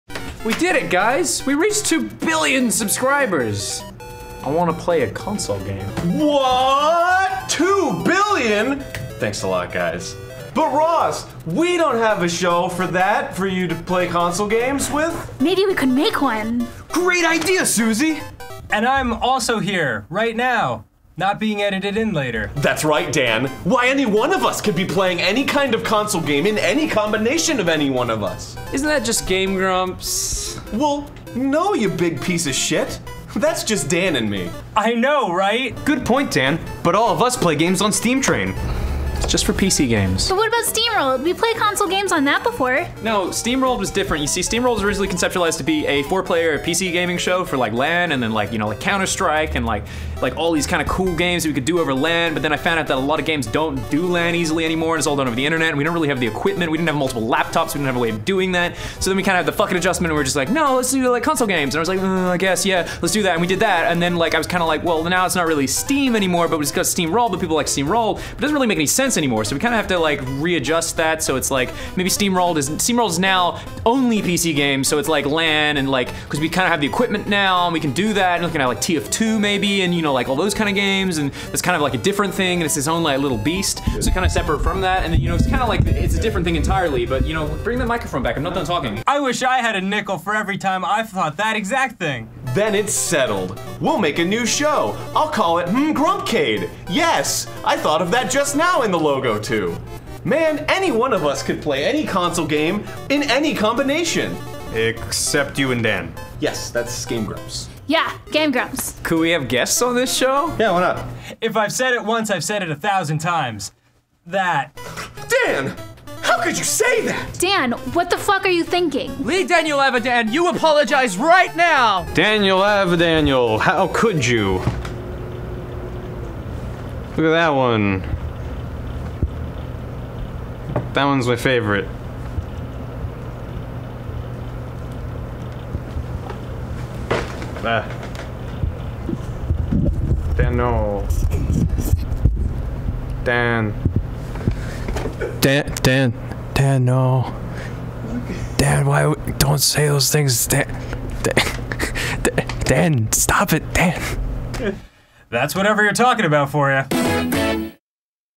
Grumpcade Intro - GrumpOut
We decided to make a new show because we like making new logos n stuff!

No, that's not a screensaver.  It's just the MitchiriNeko video playing on all three screens.